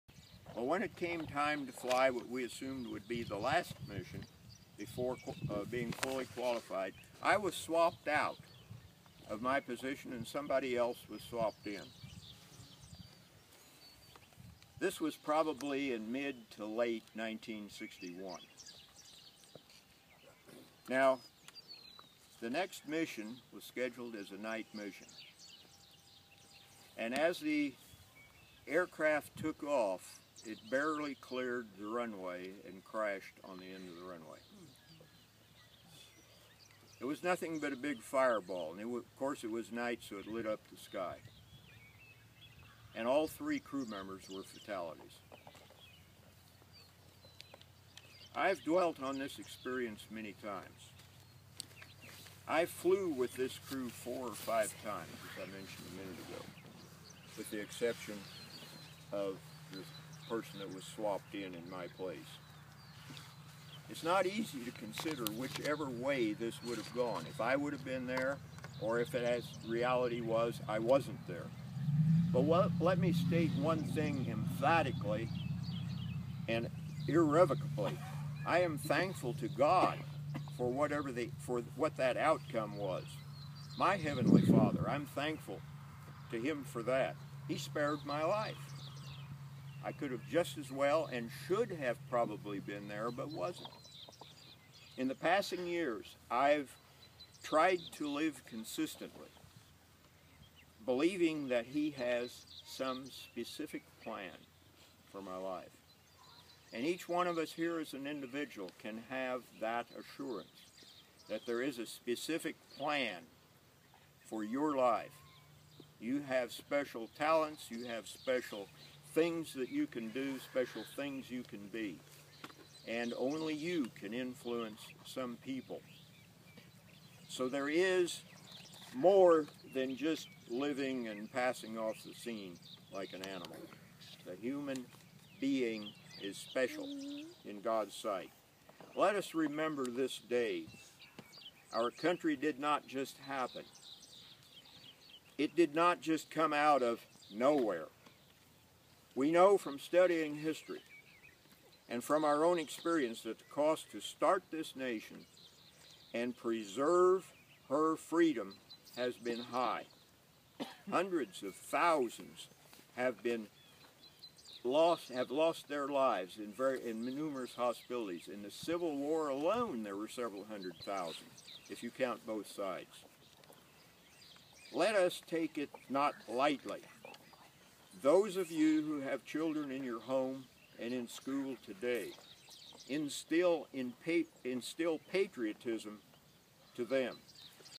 Remarks at Lewis Cemetery
Lt. Col. (ret.) Lowell Socolofsky speaks during the Memorial Day service at Lewis Cemetery near Ramona today.